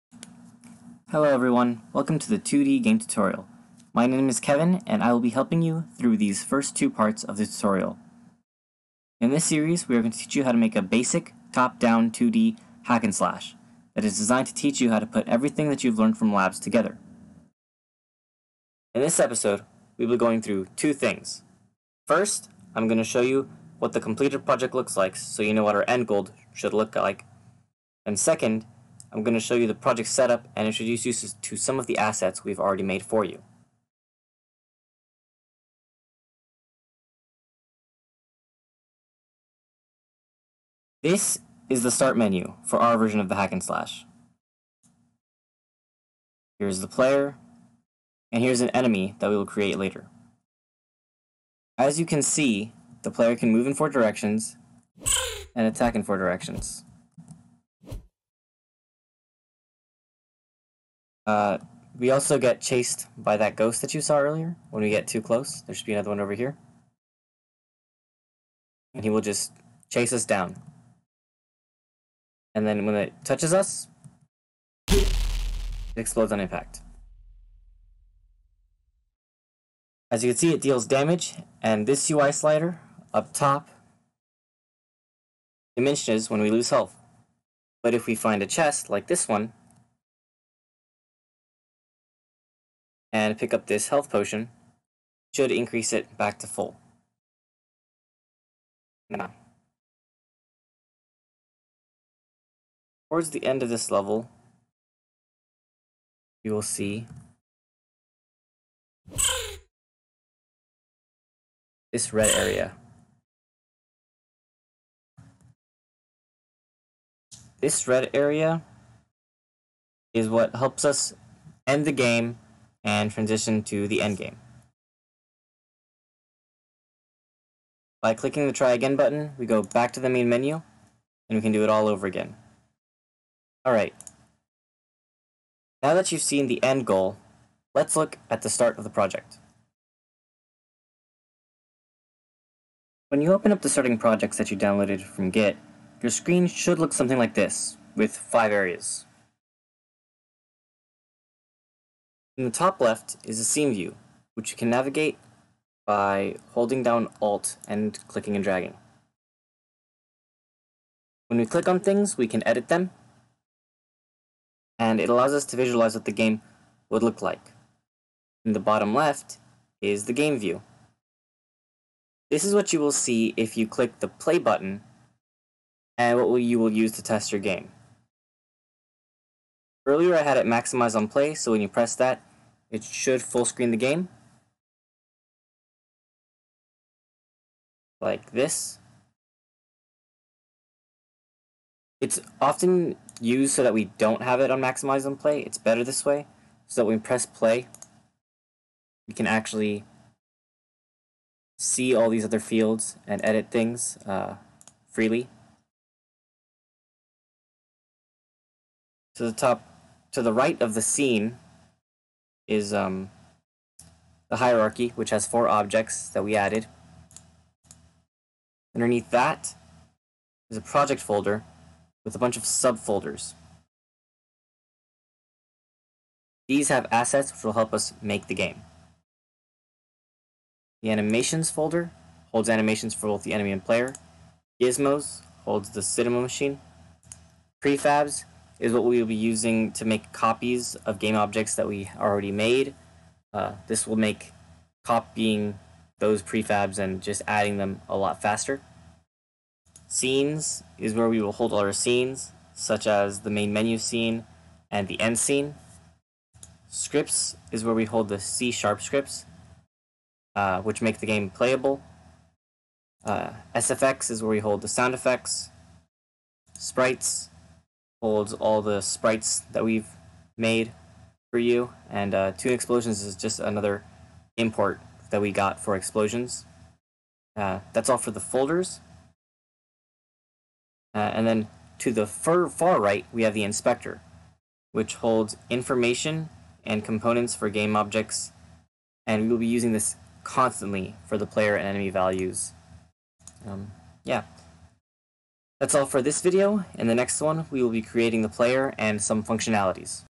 2D Game Tutorial: Episode 1, Intro
A short introduction to the 2D game tutorial. This video will show you what you will be creating, as well as walking you through the unity layout.